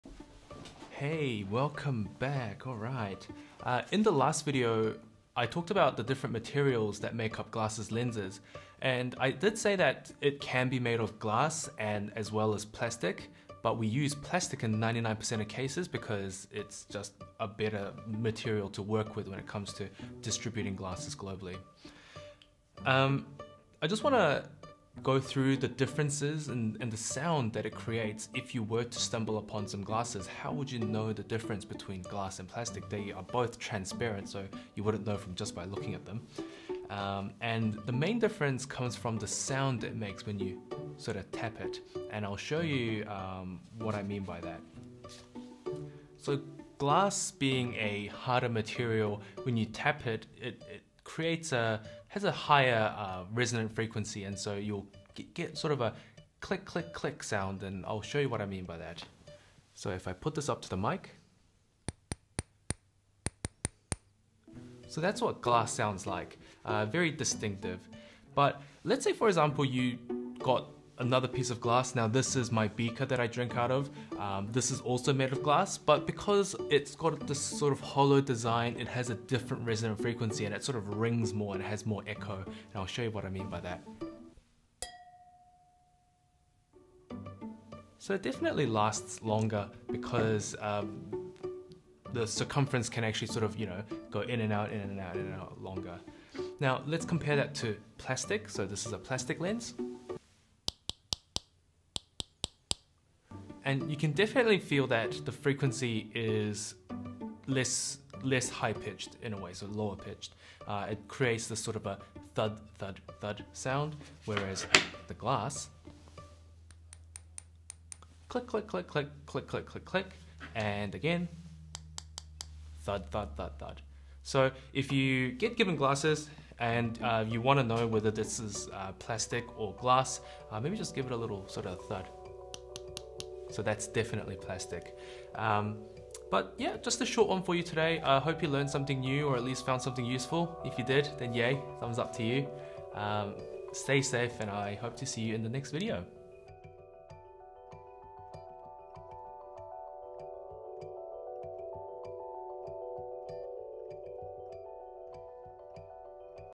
HOW to tell the DIFFERENCE between GLASS vs PLASTIC lenses | Optometrist Explains
Just a quick video for you today,

How to tell the difference between glass and plastic lenses